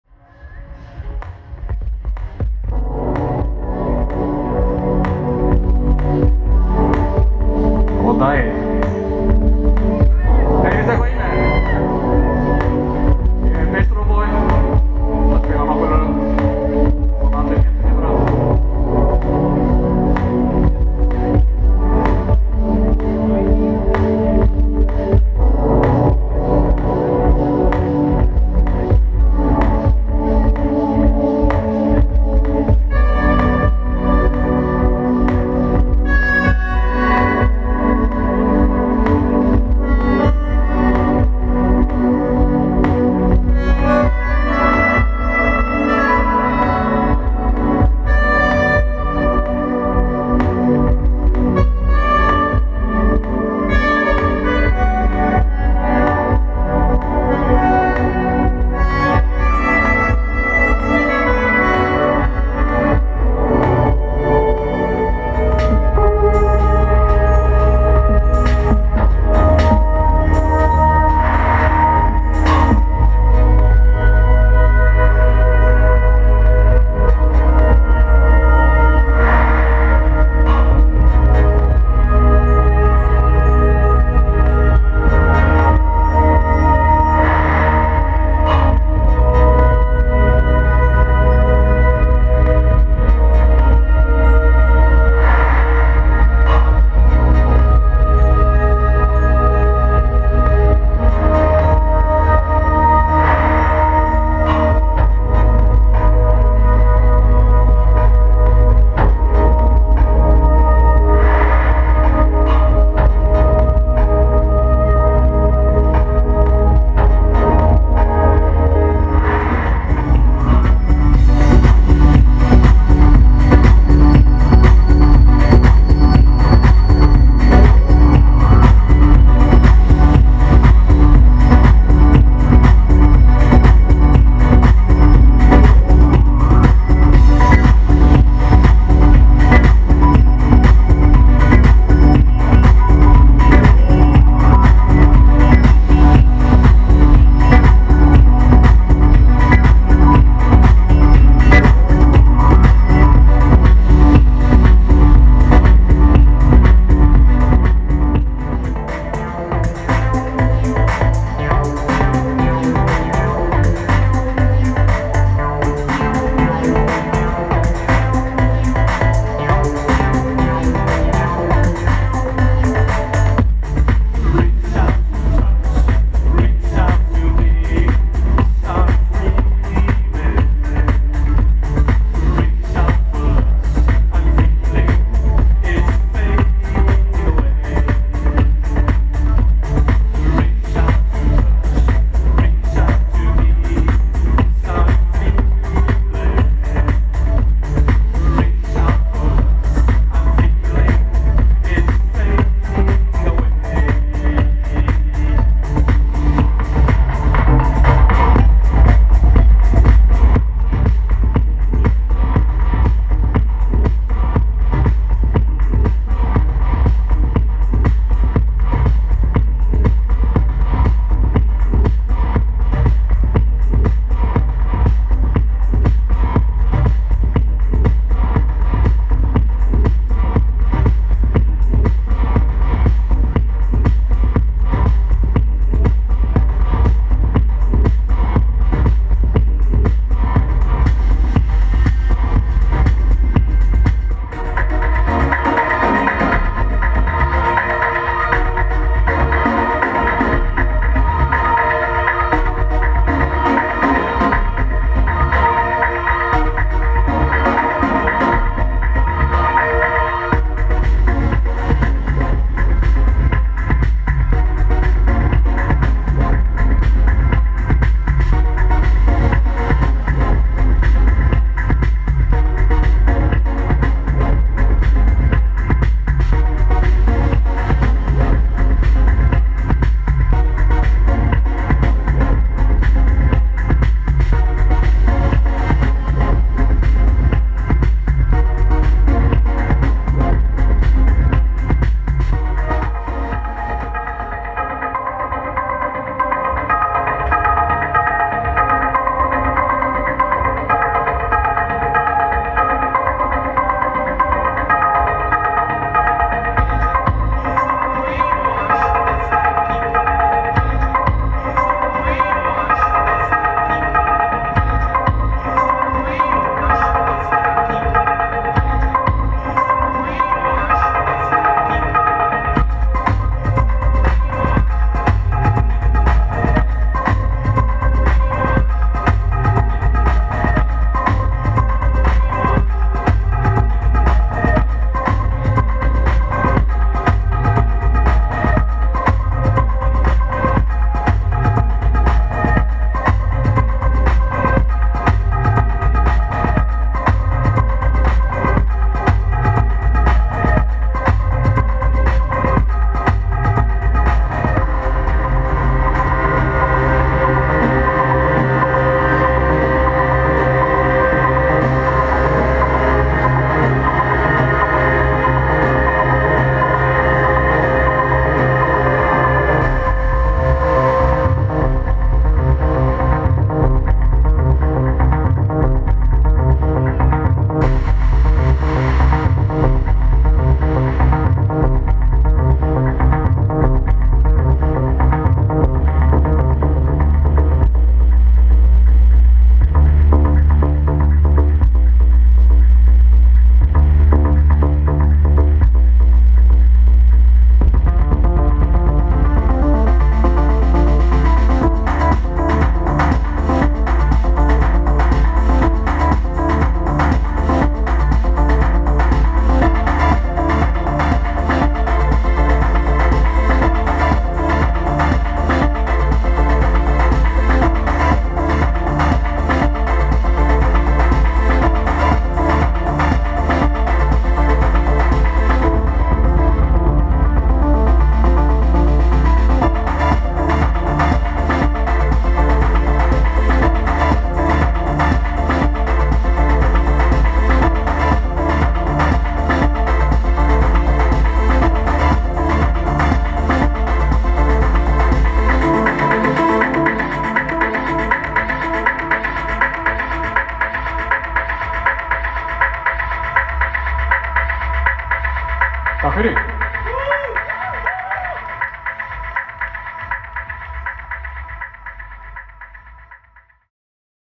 SECRET SOLSTICE 2014 | BISTRO BOY
Clips from my Secret Solstice 2014 set as seen (and heard...not the best quality) from the camera on the side.